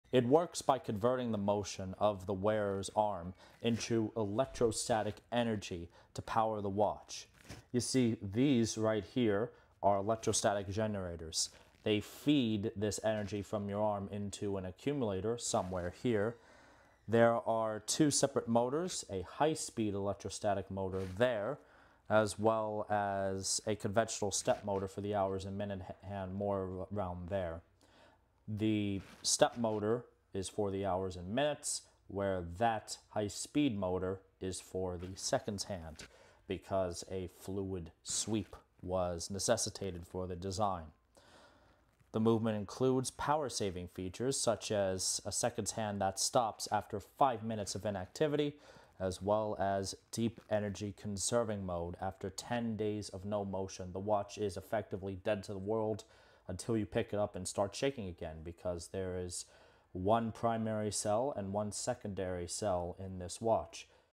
Electrostatic Motors in a Wristwatch? Yes.
No winding. No solar. The Accutron NS30 converts your movement into electrostatic power.